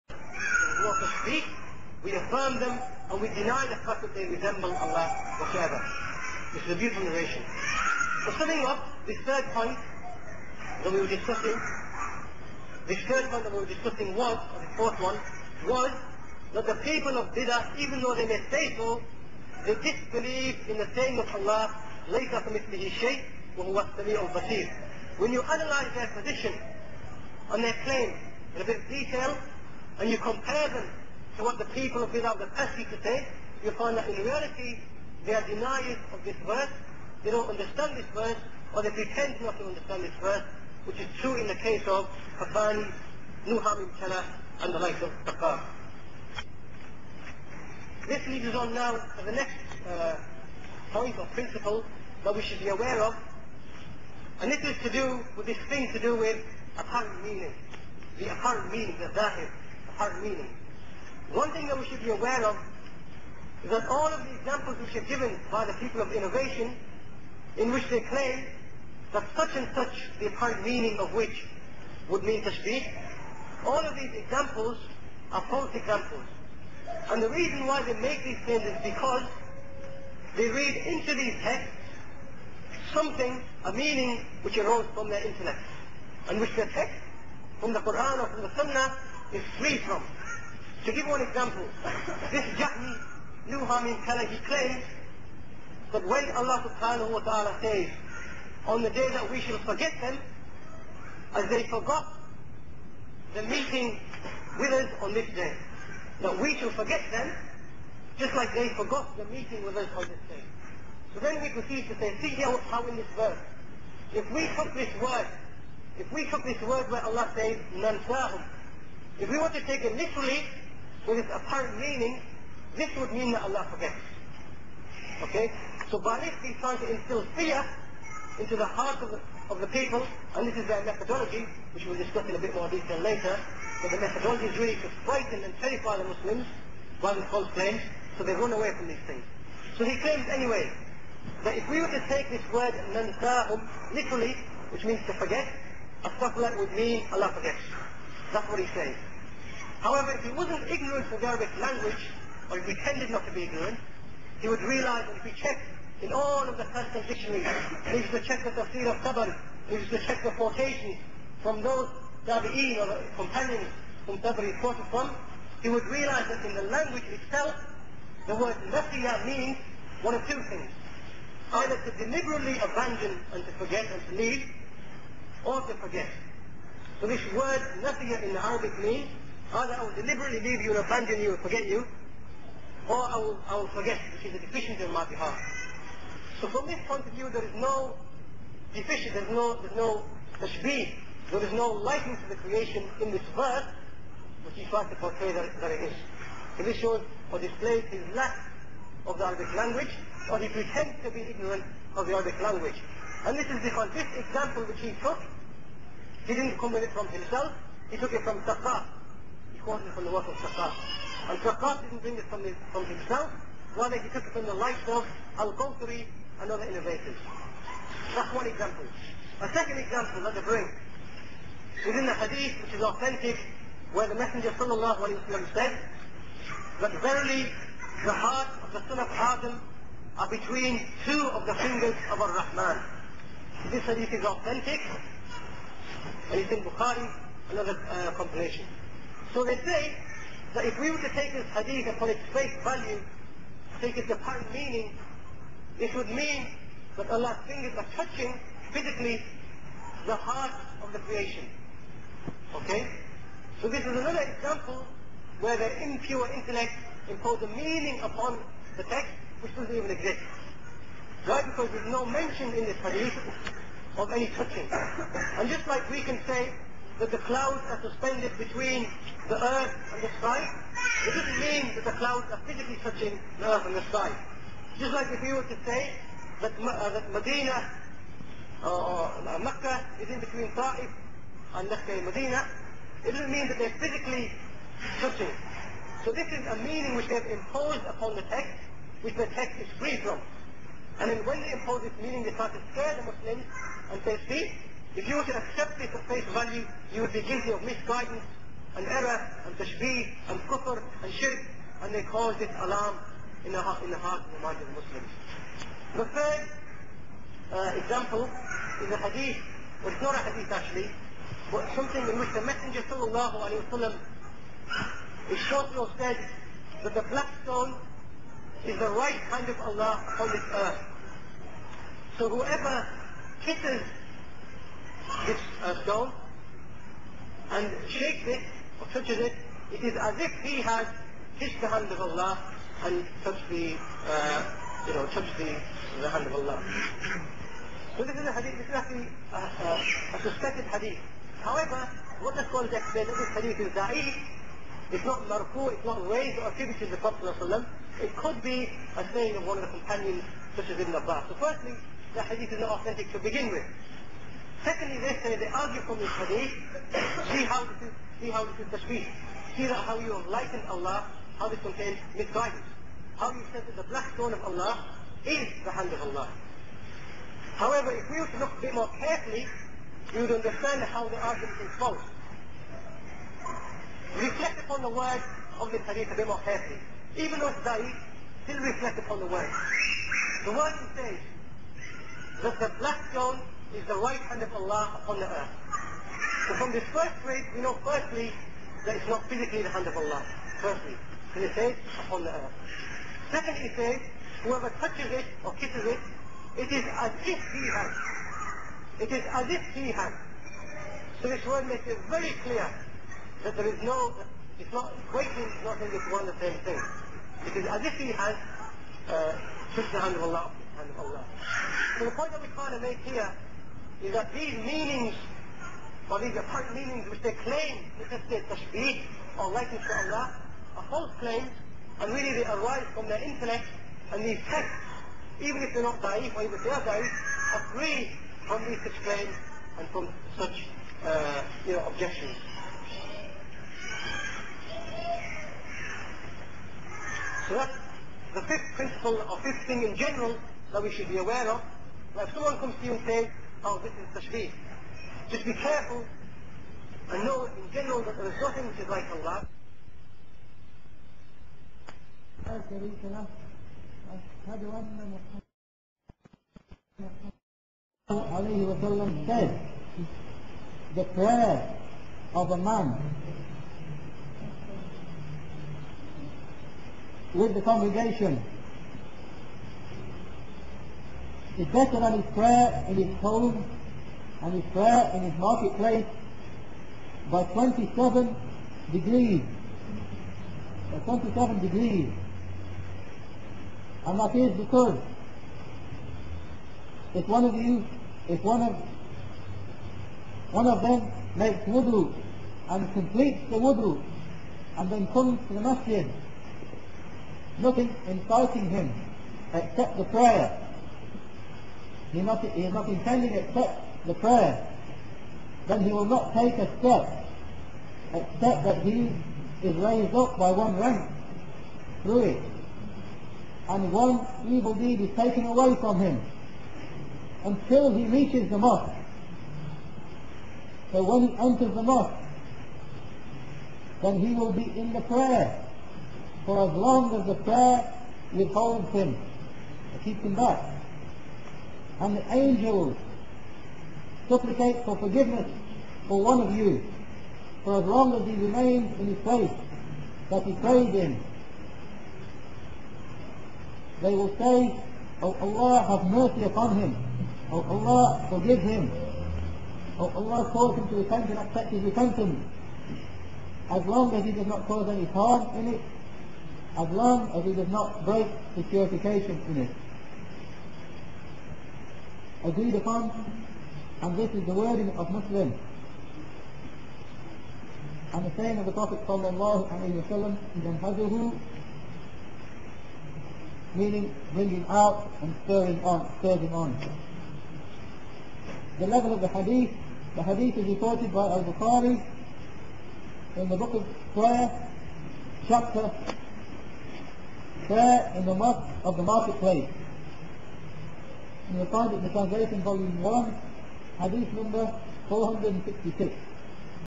The Devil Deception of Nuh Haa Meem Keller pt 4
Decisive, foundation filled refutation of the hallmarks of the Ash'ariyyah, and specifically a reply to the recycled doubts of that Filthy caller to innovation, Nooh Keller. by abu iyyaad